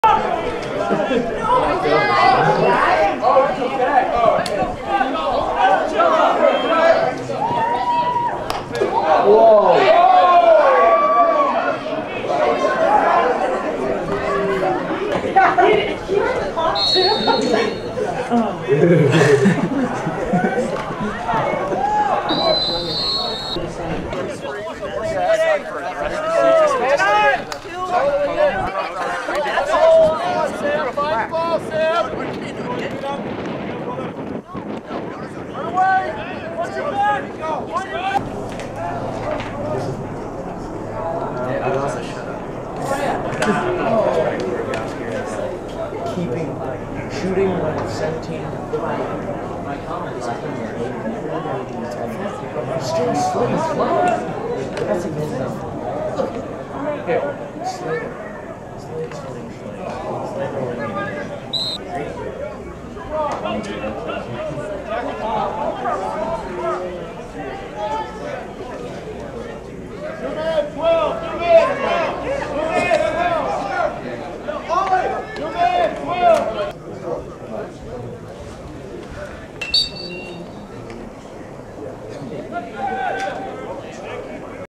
#18 Burns - Update 1
Highlights of Phillips Exeter Sophomore Sam Burns  from match-ups against Nobles, Bridgton, Cushing, Portsmouth Abbey, Pomfret, and Worcester.